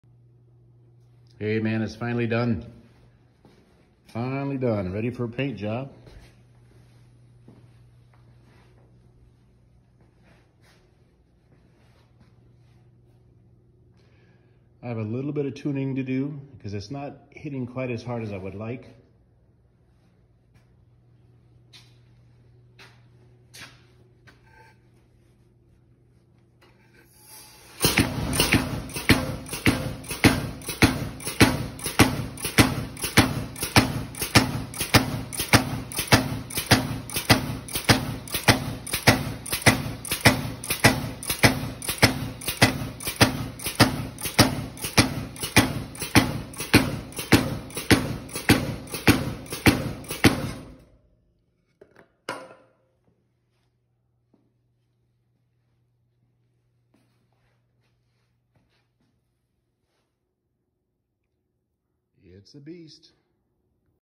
My pneumatic power hammer is DONE.
I started this build approx 6 years ago. I assembled the main frame and added the controls and air cylinder then shelved it. The supplied air in my old shop wasn’t enough to run the hammer so I stored it in a corner.
I’m in a new shop, installed my own air system and pulled this project out of the dust.
This test run is using air supplied from a hose reel. Several neck downs before the air reaches the hammer starves the pneumo. system of PSI and it hits half as fast or slower than it should run. When I run a dedicated air supply from my 3/4” shop air at 120 psi it’ll hit much faster and harder.